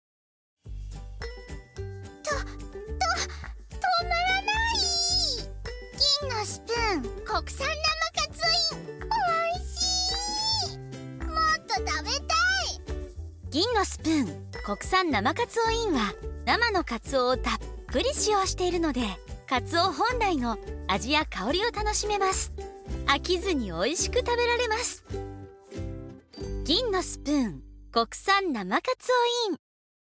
銀のスプーン国産生かつおin「と、と、とまらな～い」篇_1:1 【銀のスプーン】【国産生かつおin】【生かつお粒入り】【猫】
と、と、とまらな～い！猫ちゃん,最後のひとくちまで夢中！

生かつおをたっぷり使用した【銀のスプーン国産生かつおin】
国内で水揚げされた厳選生かつおをたっぷり使用した「生かつお粒」入り！
かつお本来の味や香りが楽しめ、猫ちゃんが、飽きずにおいしく食べられます。
「銀のスプーン　国産生かつおin」について詳しくはこちら